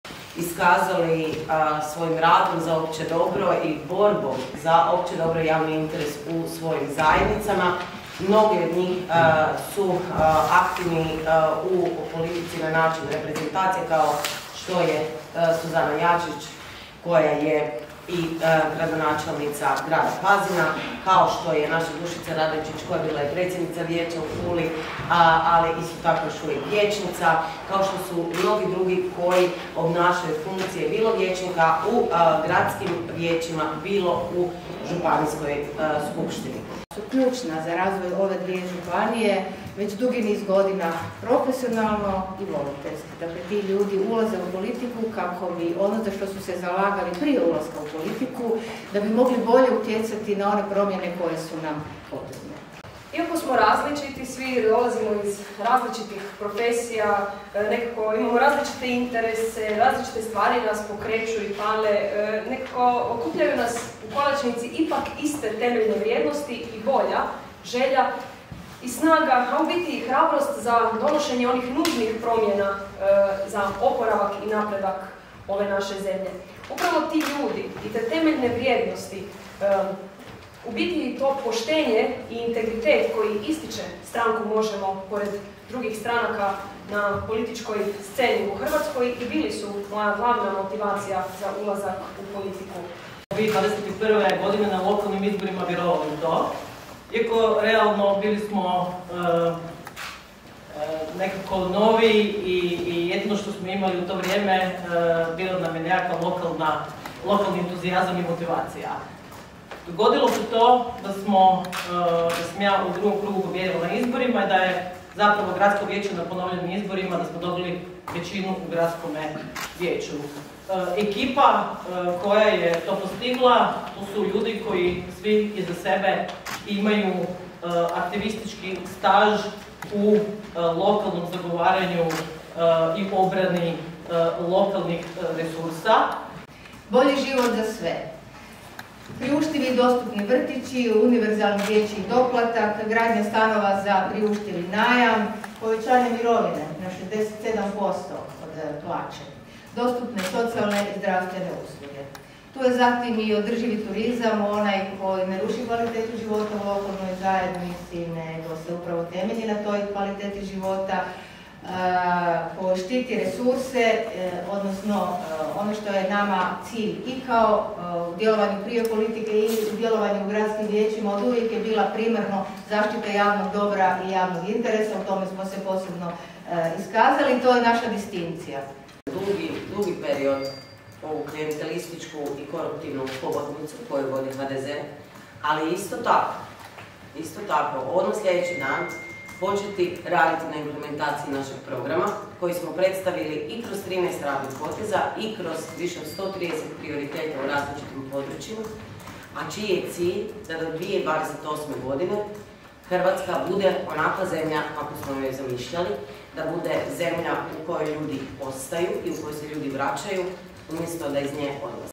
Sandra Benčić predstavila kandidate za 8. izbornu jedinicu
Na konferenciji za medije stranke Možemo! u Rijeci Sandra Benčić je predstavila kandidatkinje i kandidate u 8. izbornoj jedinici.